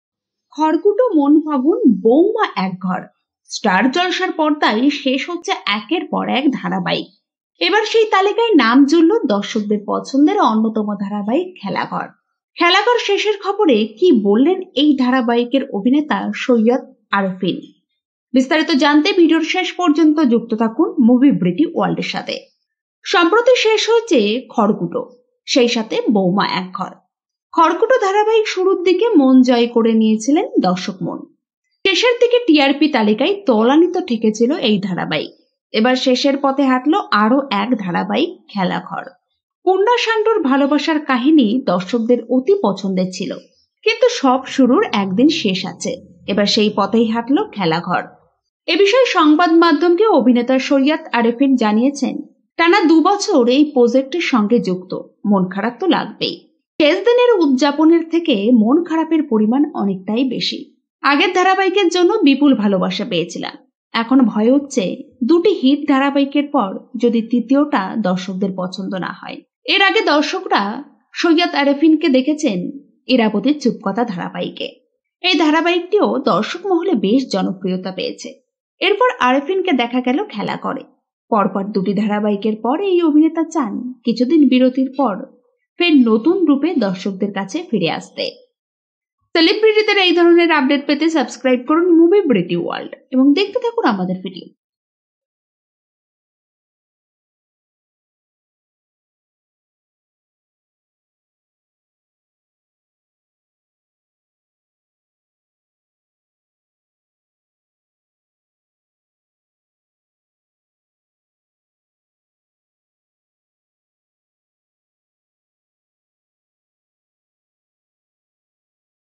শেষ হচ্ছে খেলাঘর | khelaghor | syed arefin
About the video:
সম্প্রতি শেষ হয়েছে খড়কুটো সেই সাথে বৌমা এক ঘর। খড়কুটো ধারাবাহিক শুরুর দিকে মন জ্য করে নিয়েছিল দর্শক মন। শেষের দিকে টিআরপি তালিকায় তলানিতে ঠেকেছিল এই ধারাবাহিক। এবার শেষের পথে হাটল আরোও এক ধারাবাহিক, খেলাঘর। পূর্না শান্টুর ভালোবাসার কাহিনীও দর্শকদের অতি পছন্দের ছিল। কিন্তু সব শুরুর একদিন শেষ আছে এবার সেই পথেই হাটল খেলাঘর।